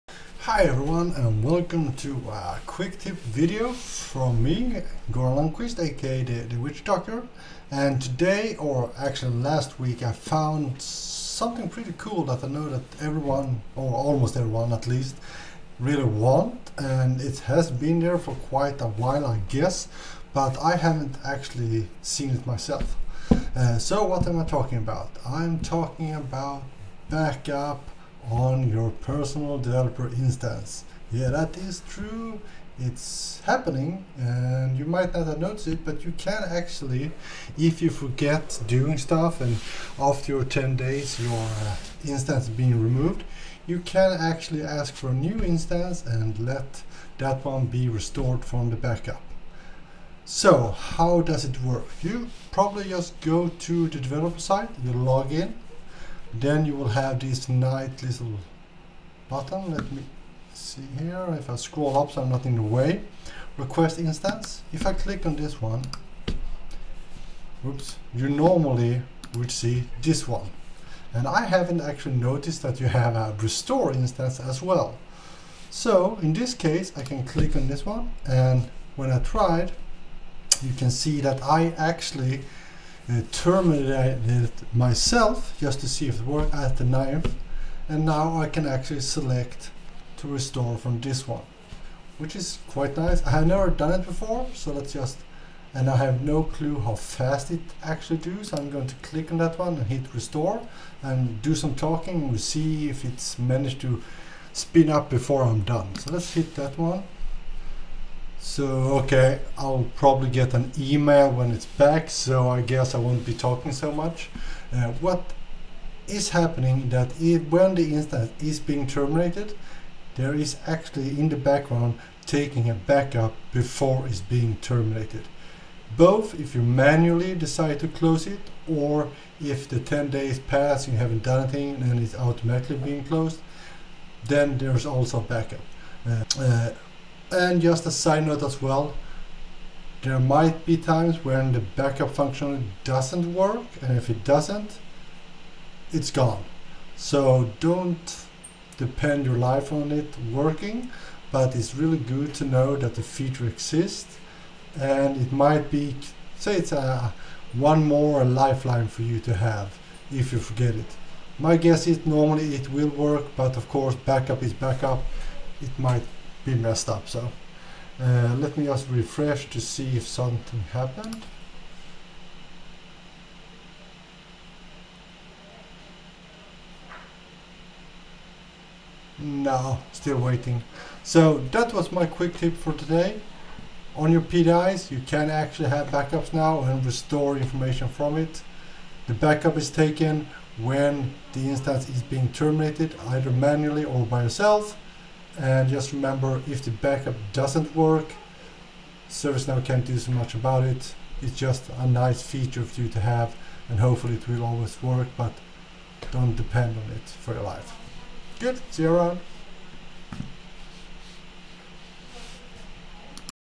Backup on your Personal ServiceNow Developer Instance is here
I recently just found out that a wanted functionality about having backup on your PDI now actually exists. Take a look and see how you can use it.